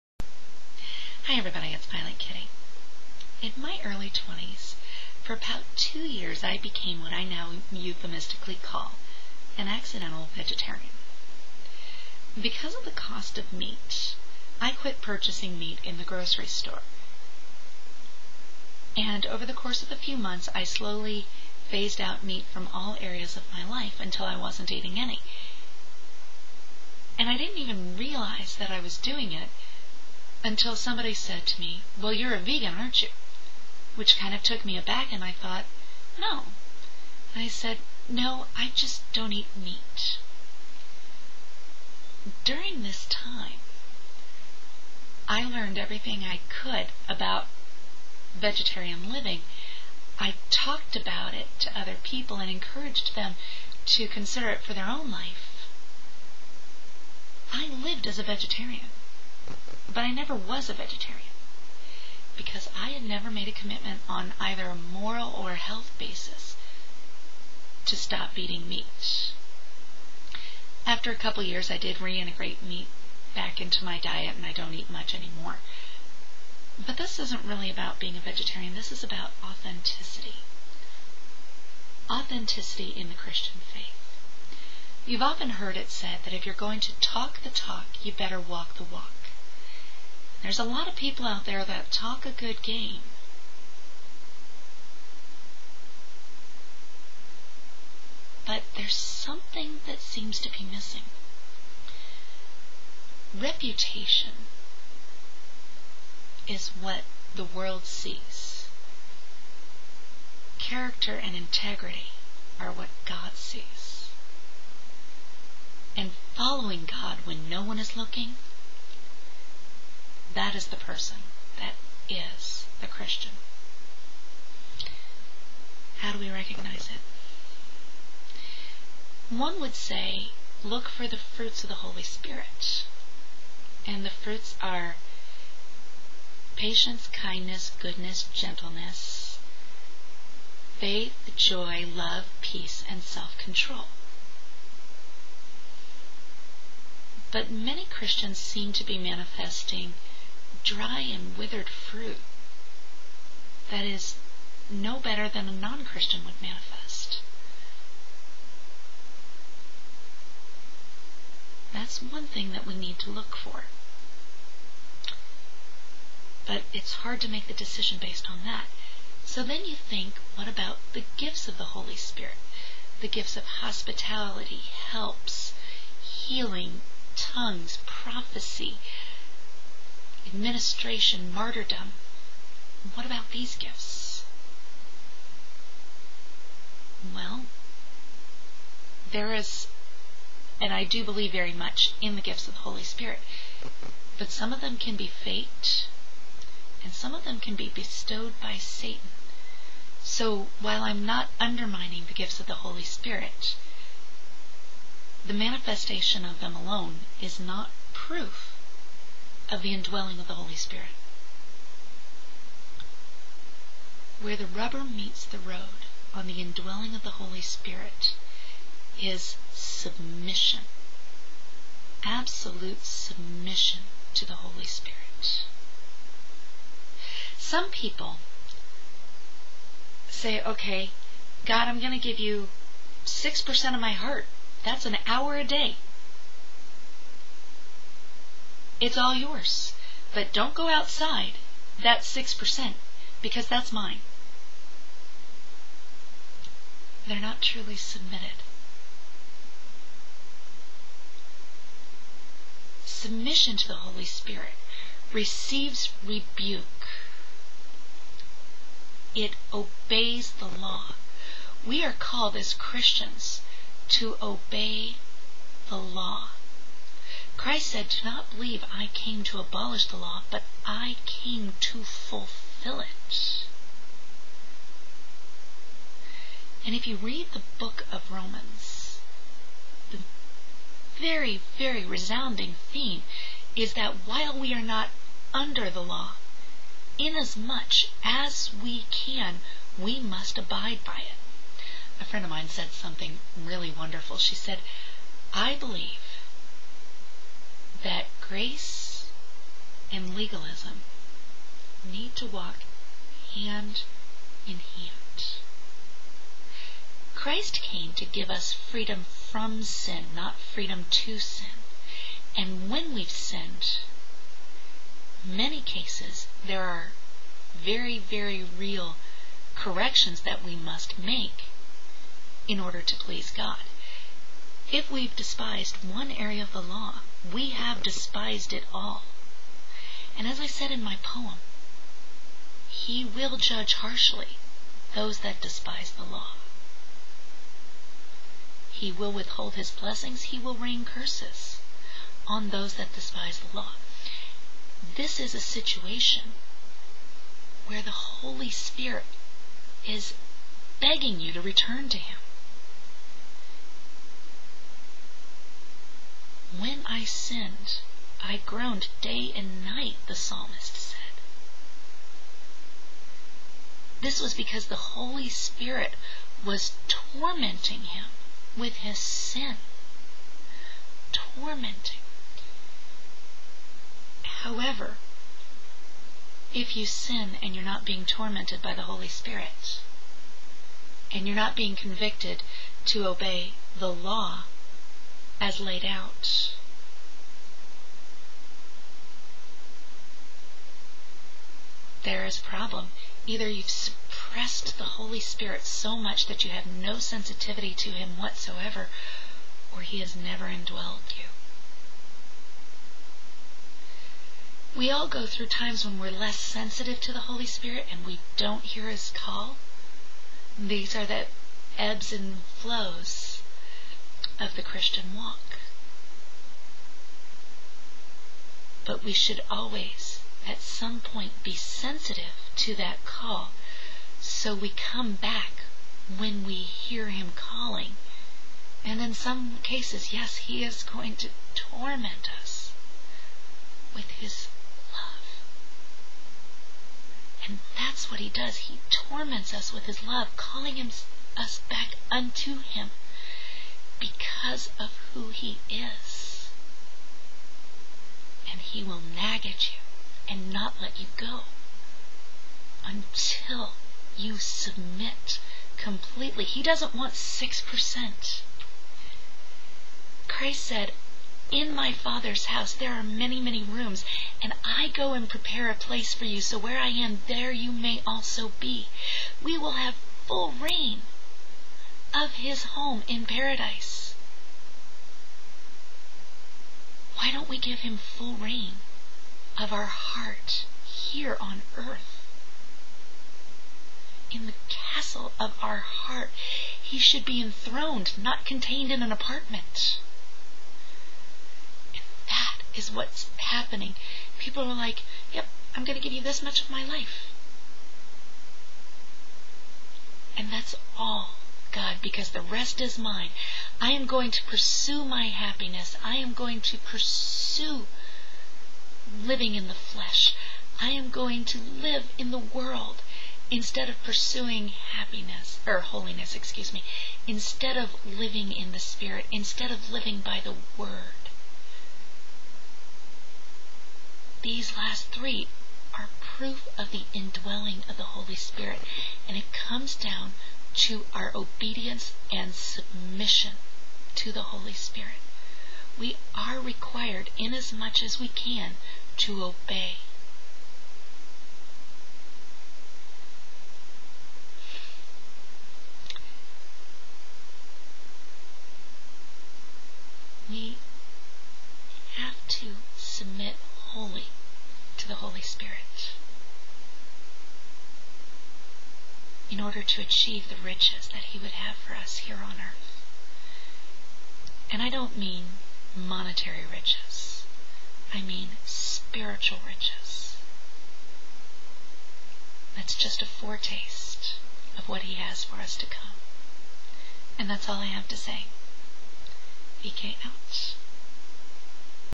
The Indwelling
Some thoughts on the Holy Spirit and how to recognize Him in ourselves.